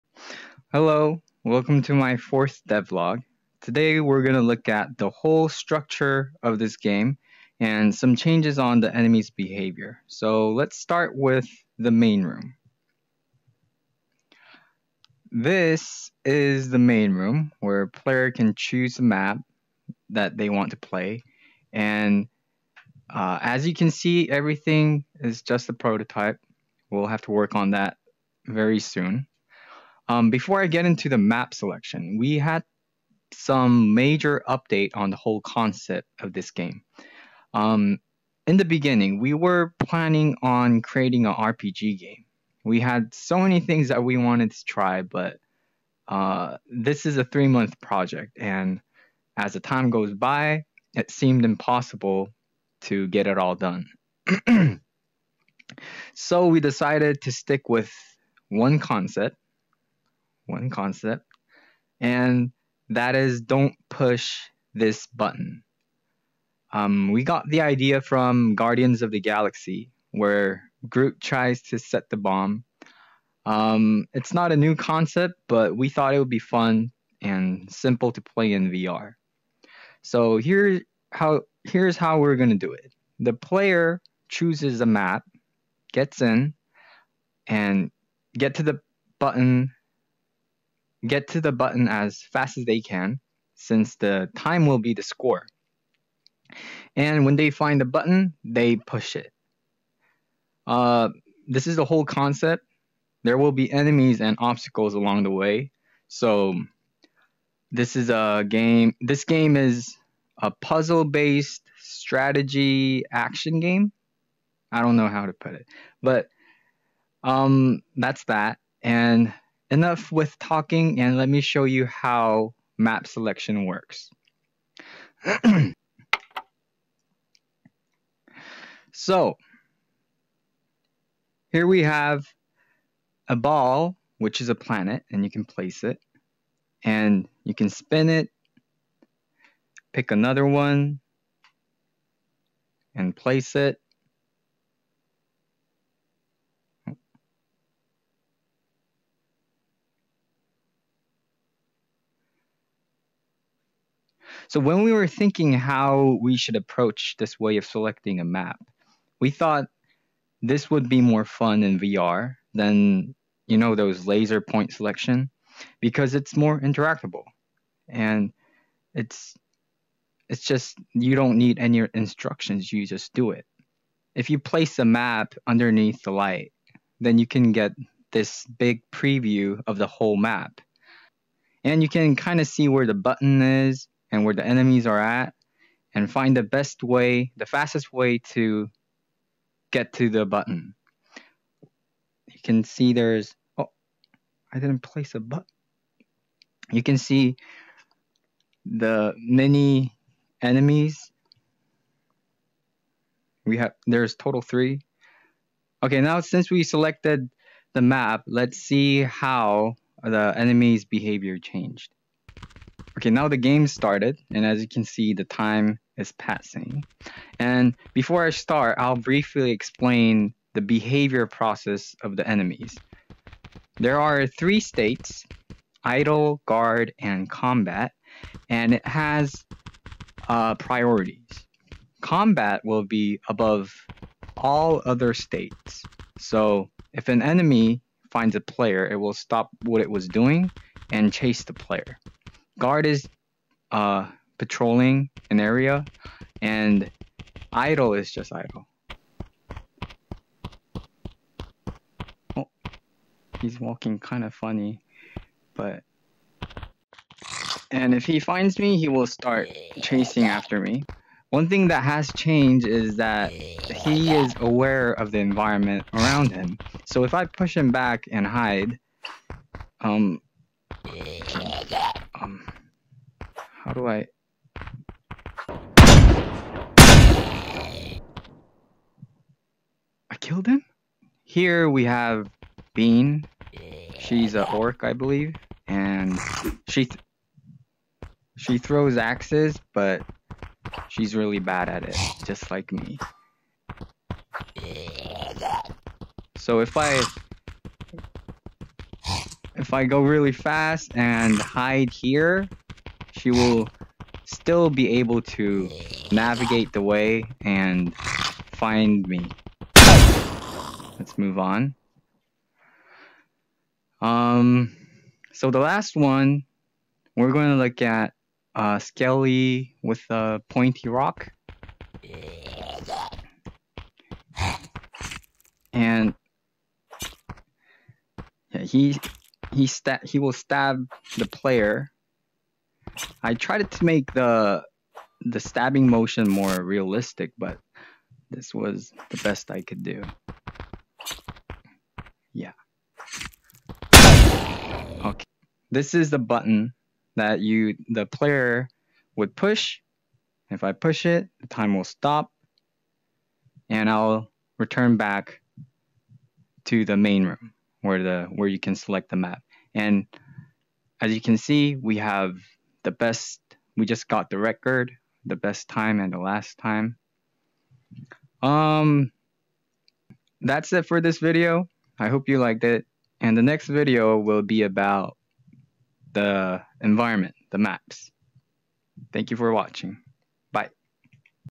New Way of Map Selection & Active Ragdoll Behaviors in Unity | Game Devlog #4
New way of map selection in VR and active ragdoll enemies behavior state.